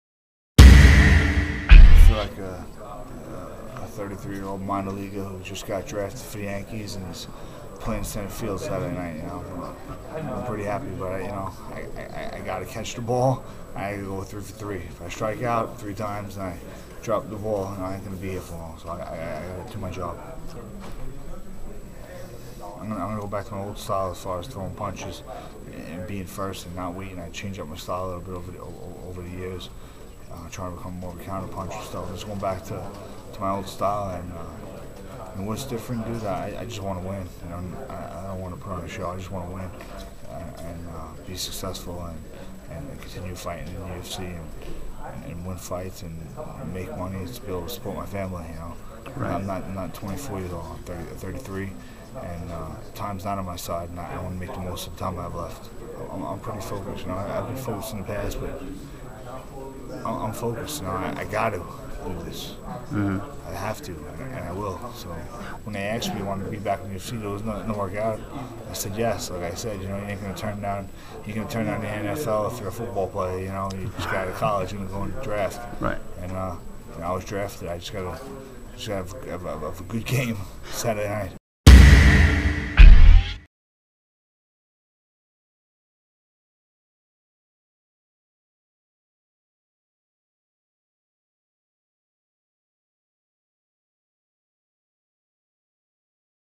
Phil Baroni returns to the UFC, talks about being a new man
Phil Baroni returns to the UFC, talks about being a new man for his fight against Amir Sadollah at UFC 106